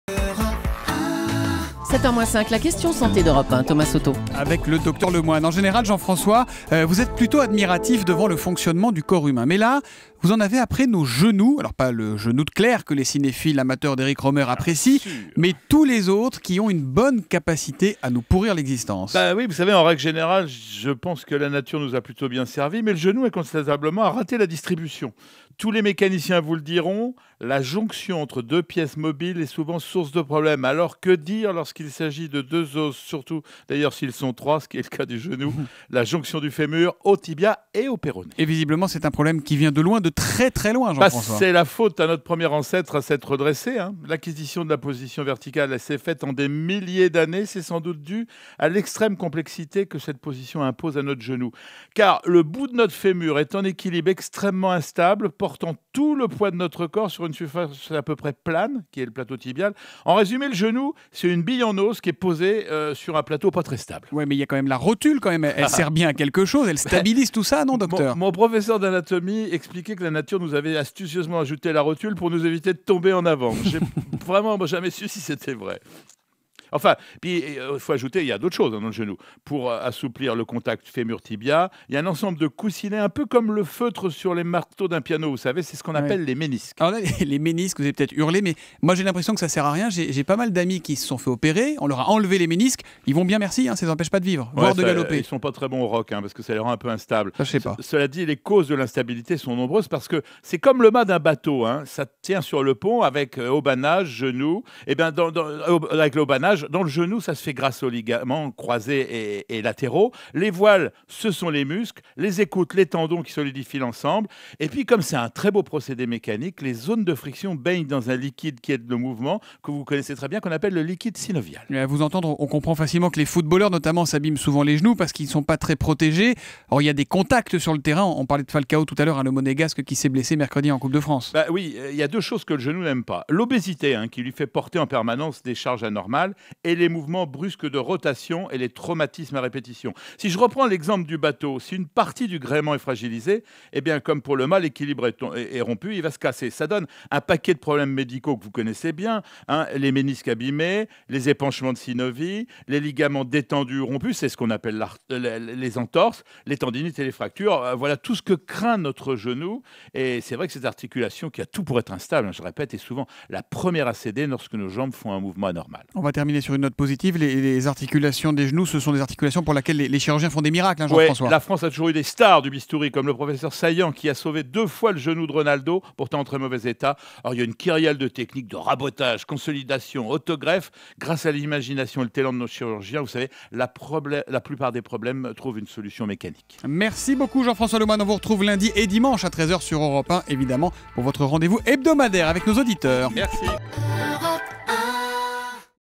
La Question Santé - Le genou, à l'origine de tous les maux.
A la jonction du fémur, du tibia et du péroné, le genou est une articulation instable et propice aux lésions. Jean-François Lemoine nous explique son mécanisme.

Retrouvez toutes les vidéos d'Europe 1 santé sur